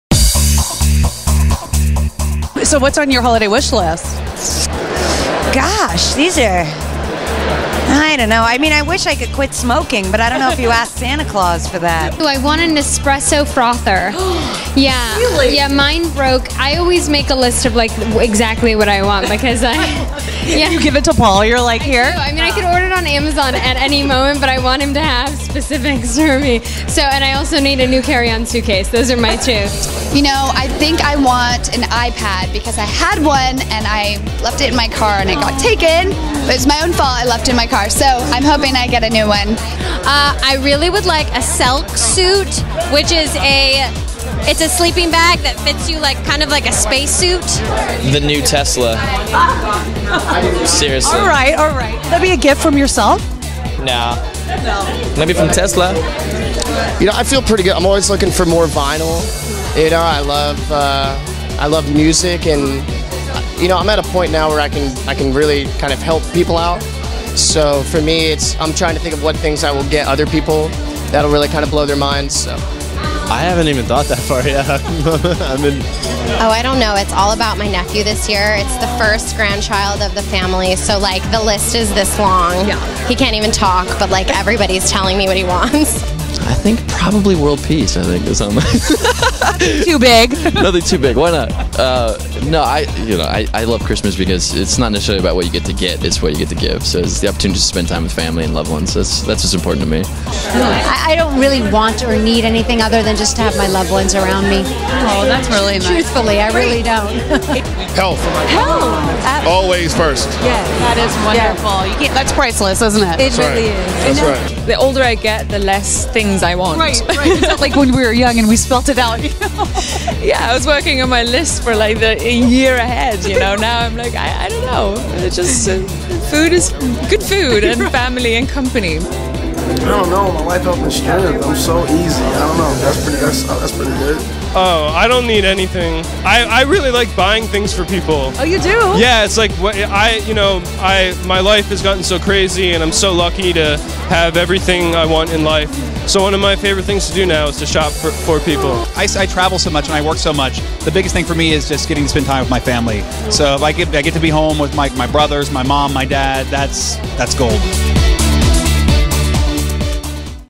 Derek Hough, Chord Overstreet, Nikki Reed and Others Talk Their Christmas Wish Lists
What do Nikki Reed, Natasha Lyonne, Derek Hough, Chord Overstreet and other celebs want in their stockings or under the tree this year? Watch this video and find out!